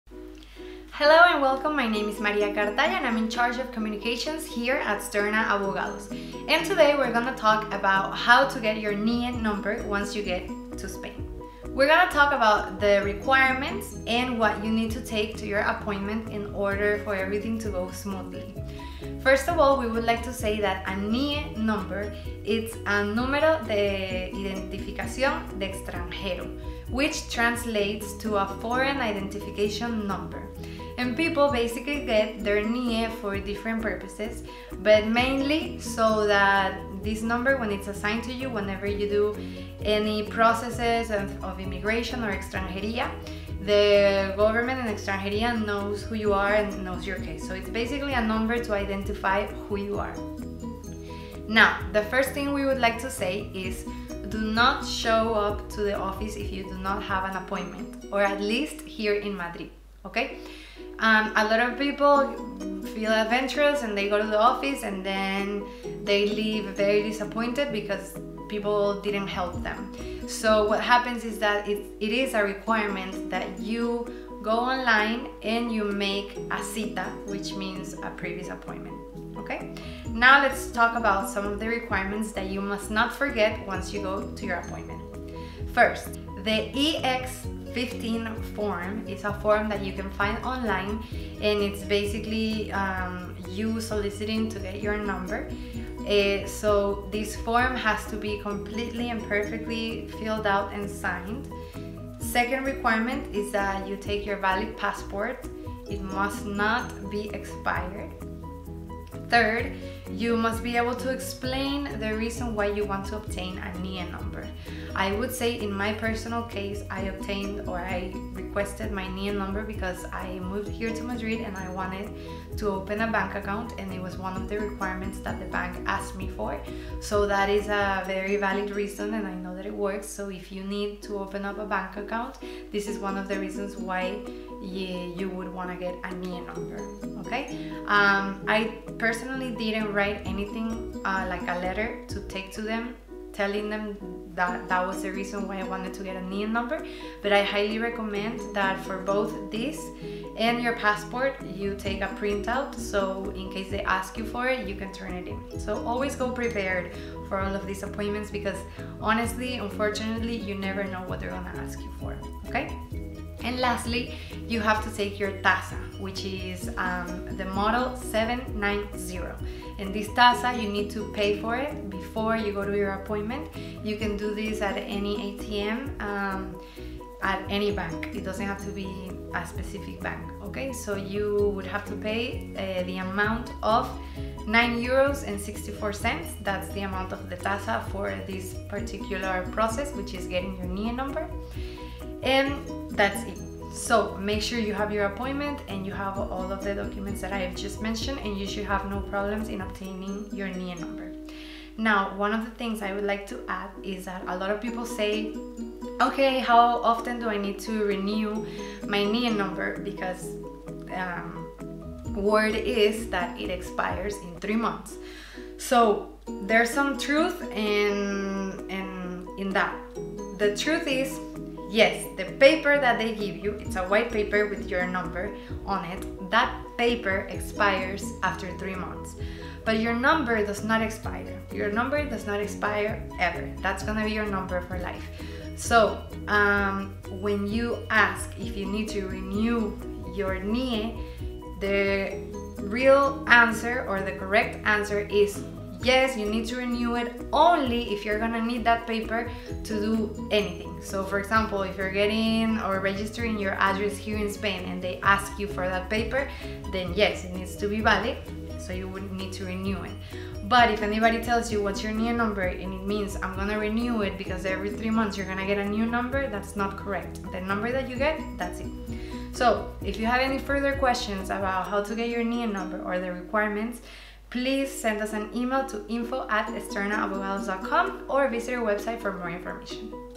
GET YOUR N.I.E IN SPAIN - Requirements 2019
In this video, we explain the process of getting a NIE number in Spain as well as the requirements and everything you need to take with you to your appointment.